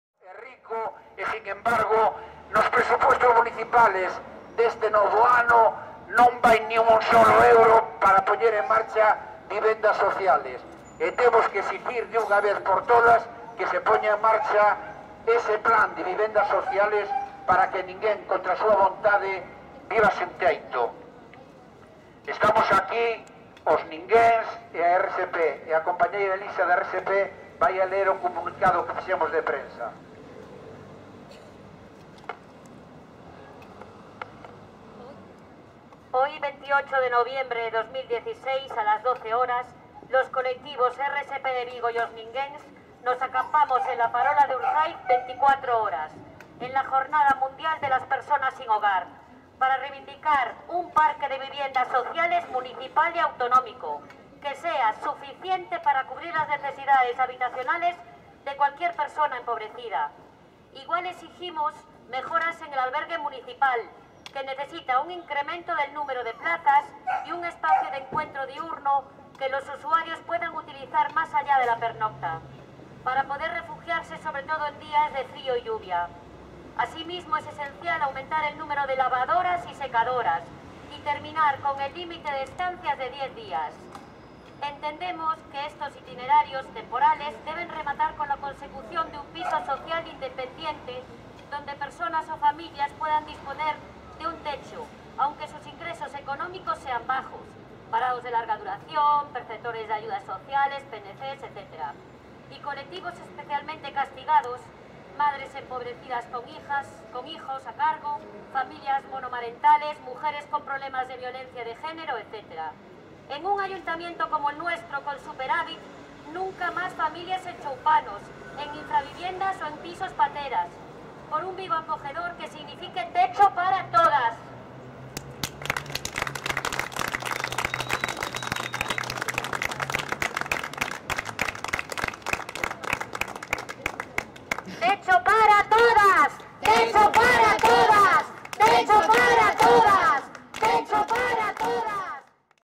Os Ninguens y la RSP, acampados en Vigo 28/11/2016
En el día mundial de los sin techo, los activistas acampan en la Farola de Urzáiz, en Vigo, para visibilizar la situación desesperada de tantas y tantas personas, abandonadas por la administración a su suerte.